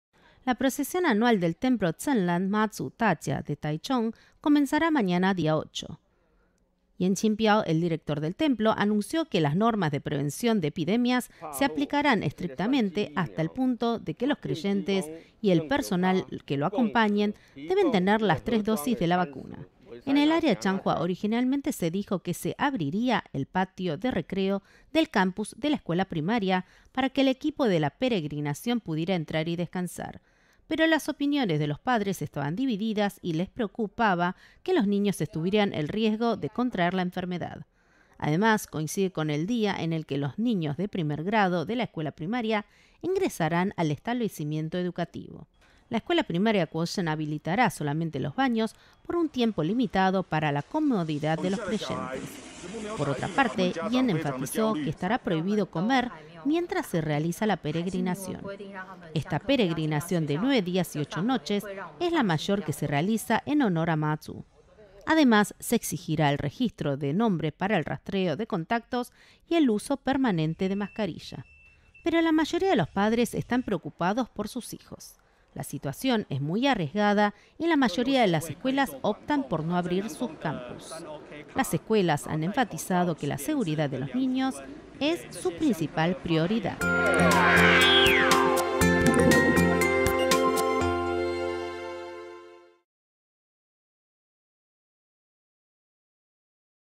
【RTI】Se inicia la procesión anual de Mazu Dajia del templo Zhenlan
La procesión anual del templo Zhenlan Mazu Dajia en Taichung comenzará mañana día 8. Yen Ching-piao's (顏清標), el director del templo, anunció que las normas de prevención de epidemias se aplicarán estrictamente, hasta el punto de que los creyentes y el personal que lo acompañen deben tener las tres dosis de la vacuna.